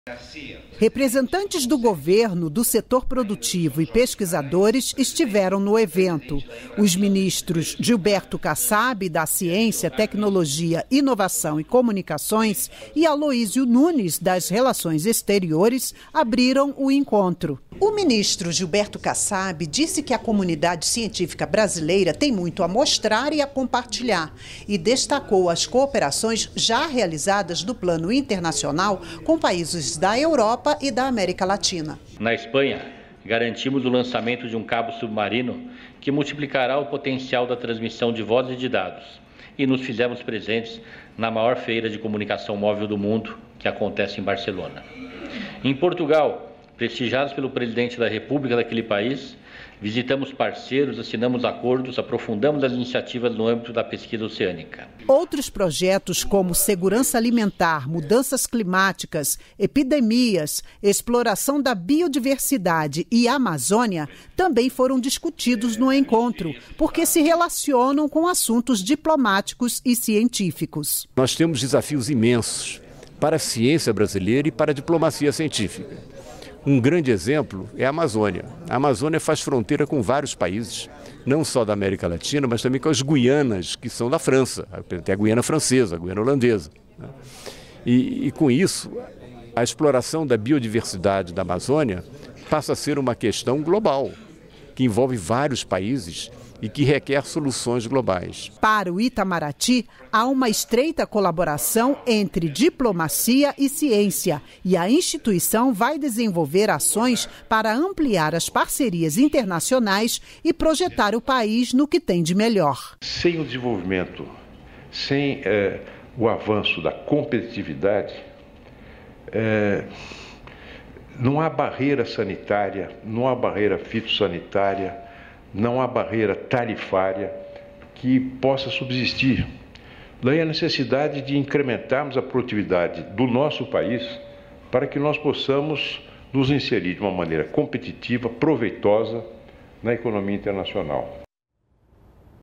Brasil discute ciência, tecnologia e inovação em seminário
REPÓRTER NBR - 28.09.17: A atuação do Brasil nas instituições internacionais e a cooperação com outros países em áreas como  ciência, tecnologia e inovação foram discutidos em um seminário, em Brasília. O ministro Gilberto Kassab participou do encontro e destacou três assuntos que são considerados prioritários para o país: estratégia digital; segurança cibernética; e governança da internet.